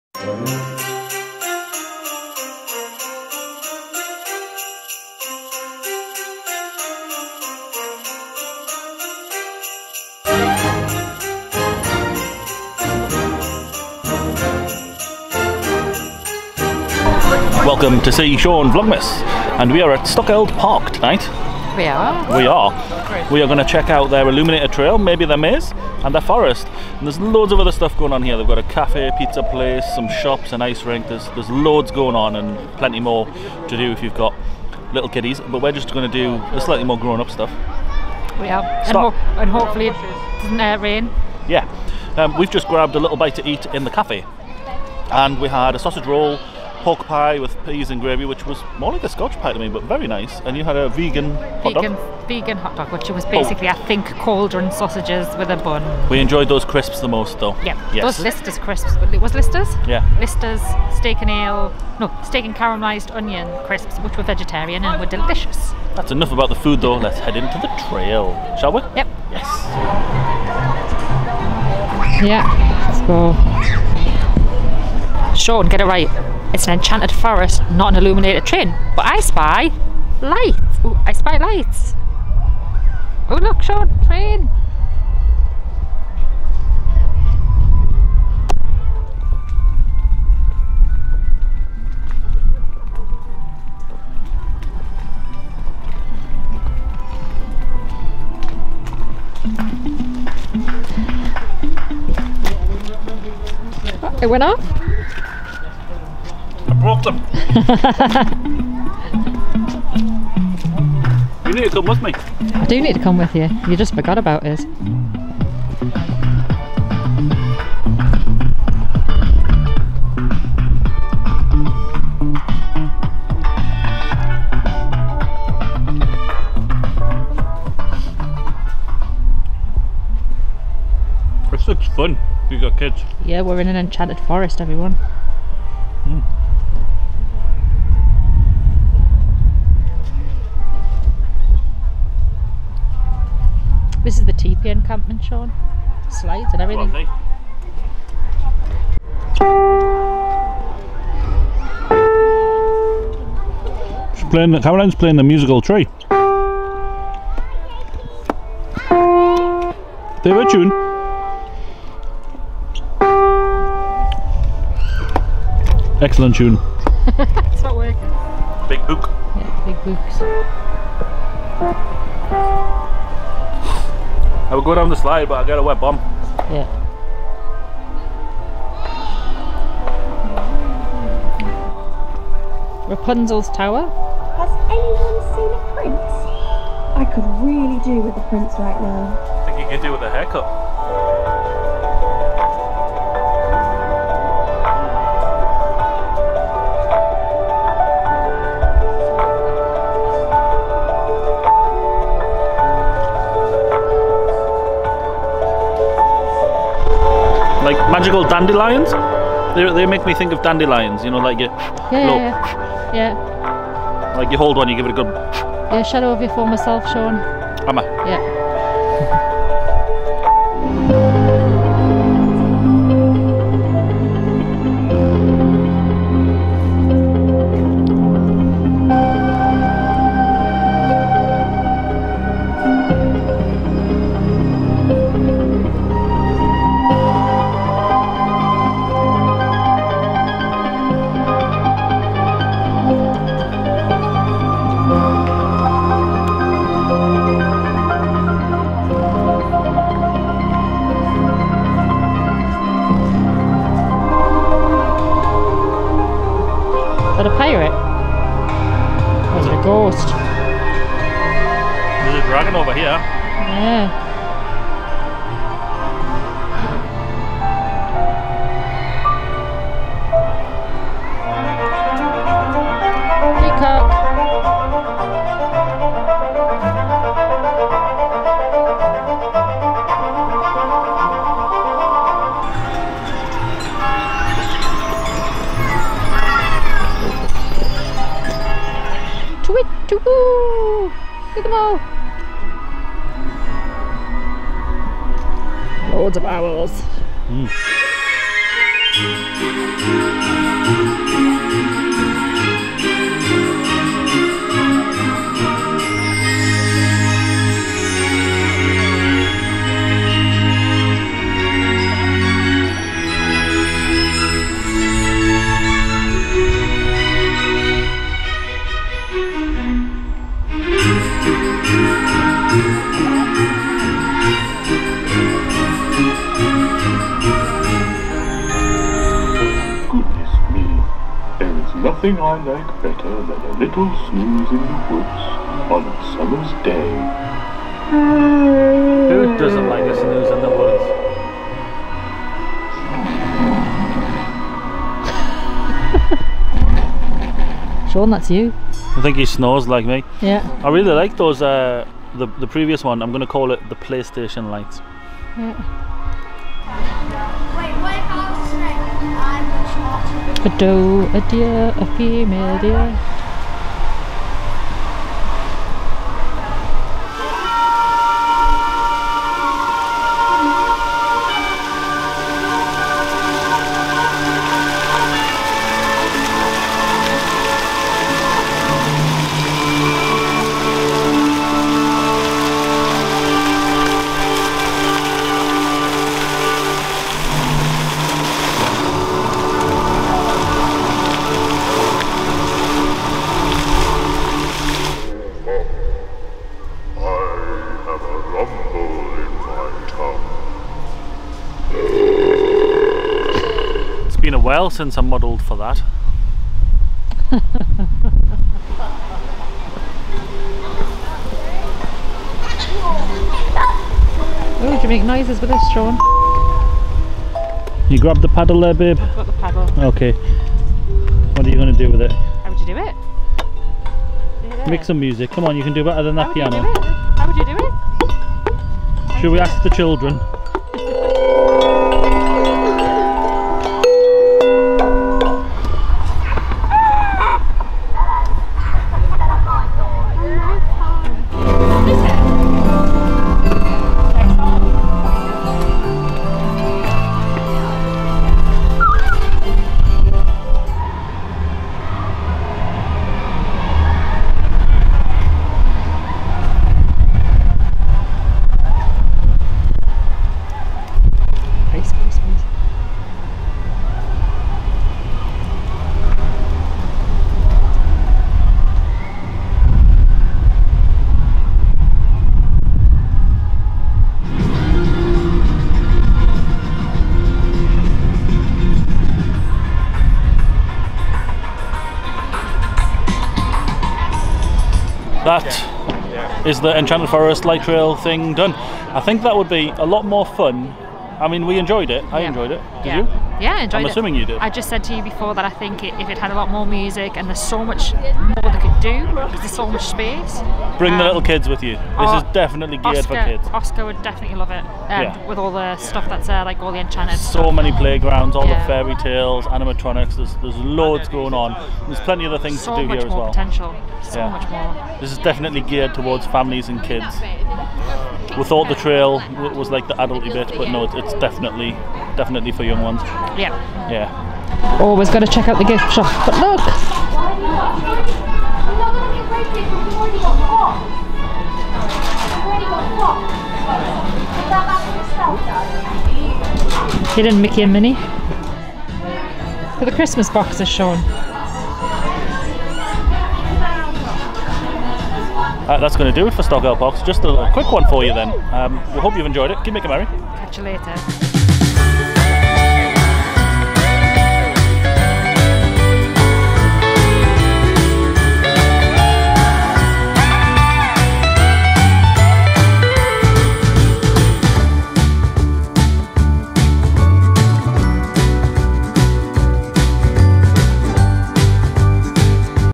Illuminated Christmas Trail at Stockeld Park - Vlogmas 2023
We head to Stockeld Park near Wetherby for their Christmas event. It's our first time visiting Stockeld Park and we take on the Illuminated Light Trail.

Shop - shop.spreadshirt.co.uk/seeshaunvlog